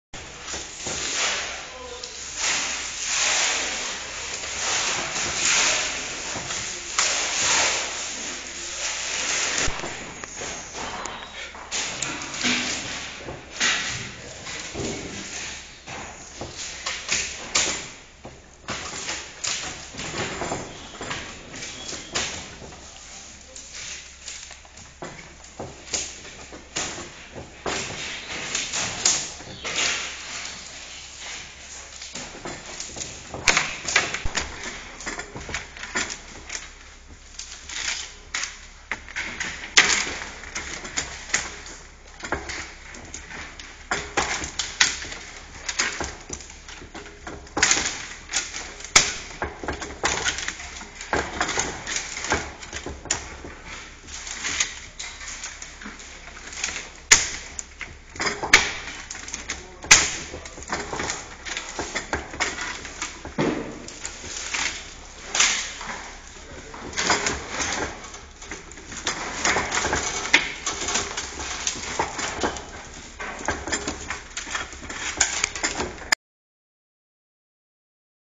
Making a Hurdle
John making a hurdle at English Hurdle